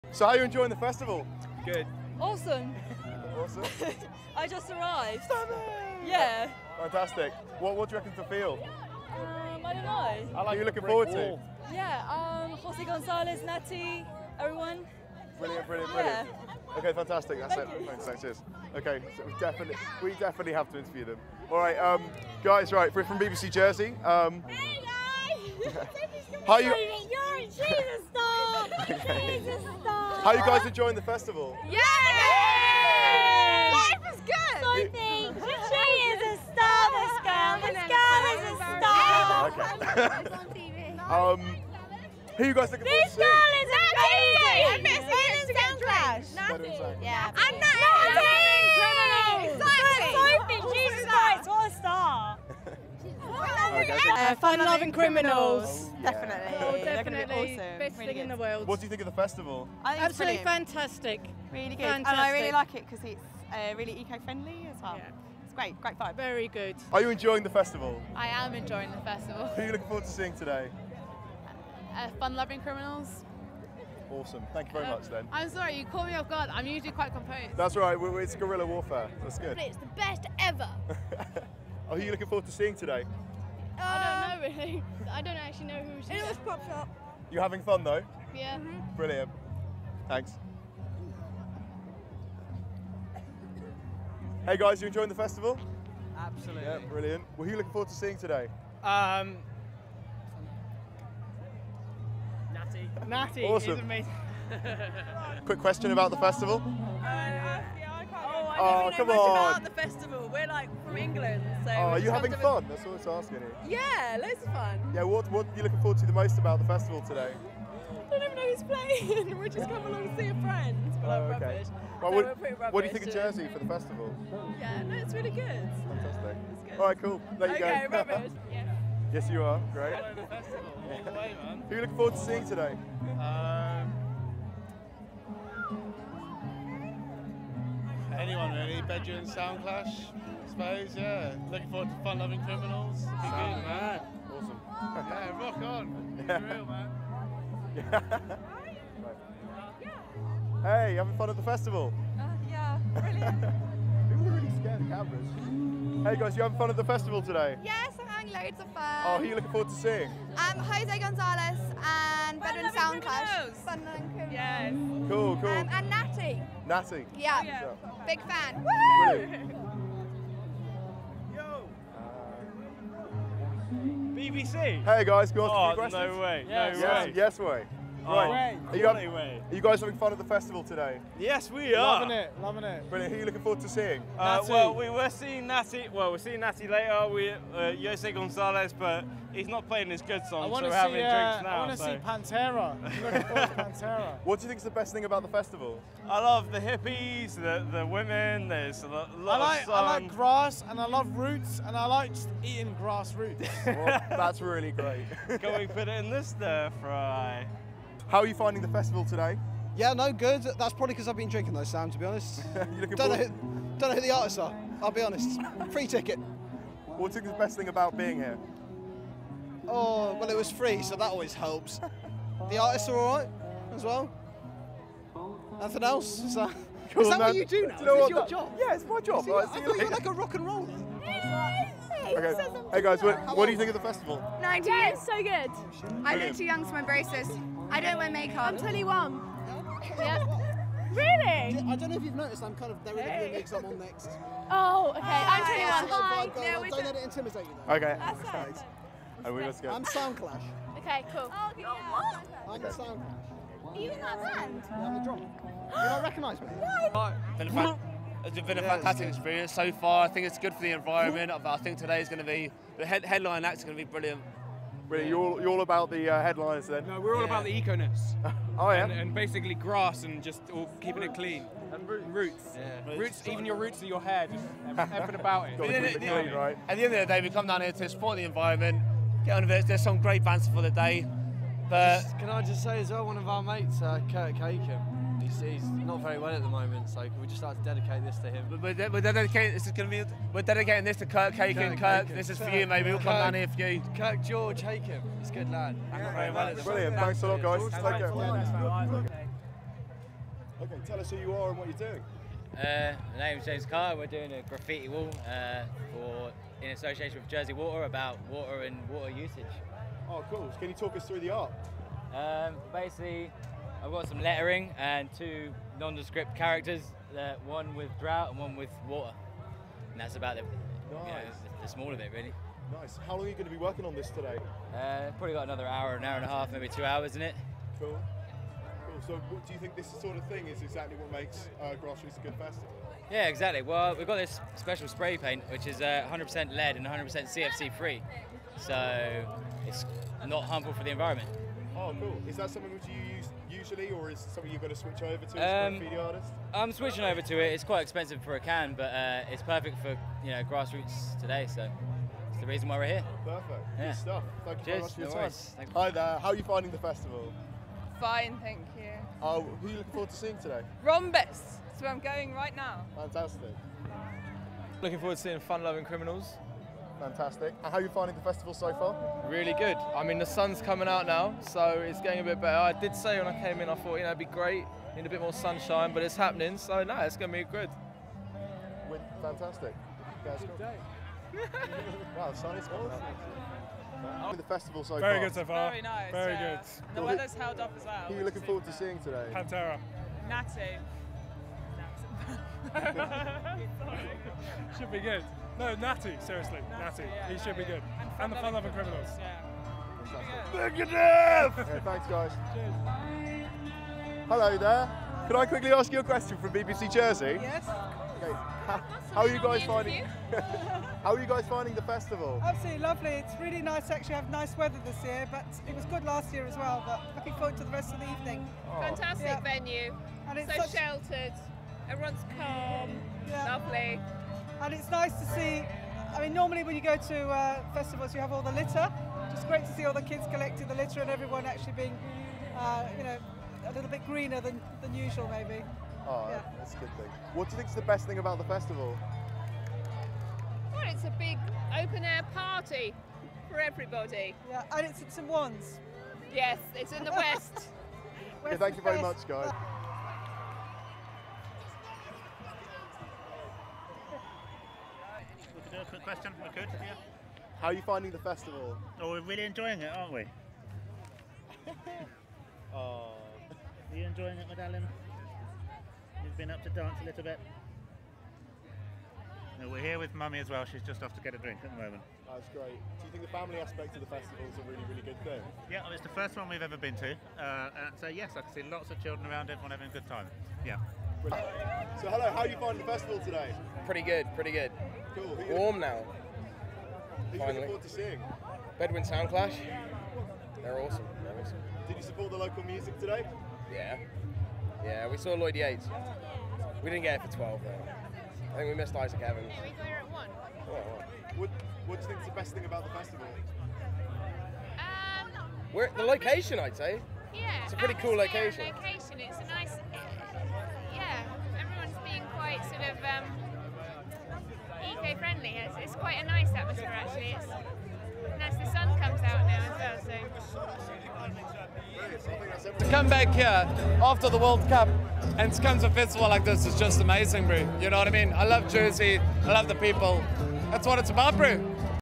The Grassroots festival ambush
BBC Blast Introducing reporter Sam Falle drags BBC Jersey Introducing presenter and camera around the Grassroots festival asking people their thoughts on the 2010 event.